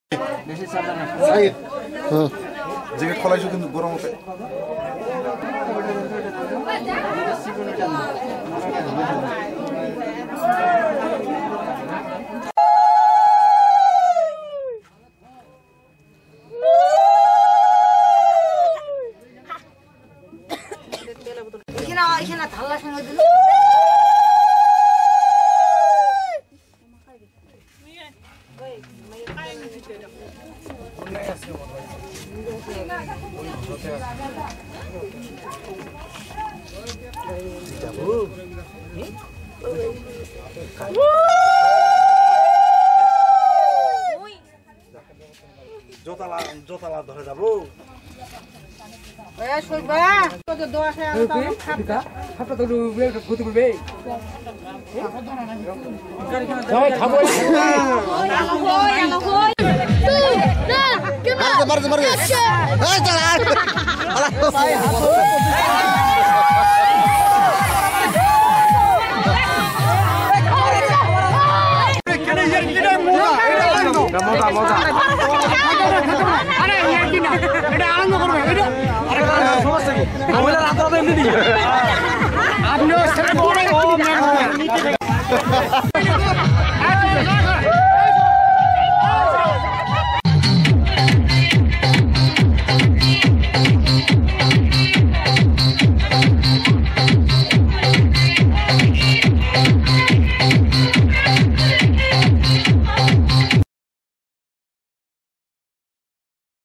Hindu Biye bari | হিন্দু বিয়ে দেখলে অবাক হবেন | যারা দেখেননি তারা দেখুন / না দেখলে মিস করবেন
এটি গ্রামের বিয়ের অনুষ্ঠানের ভিডিও । যদি ভিডিওটি ভালোলাগে তাহলে কমেন্ট করে সাথে থাকবে । ধন্যবাদ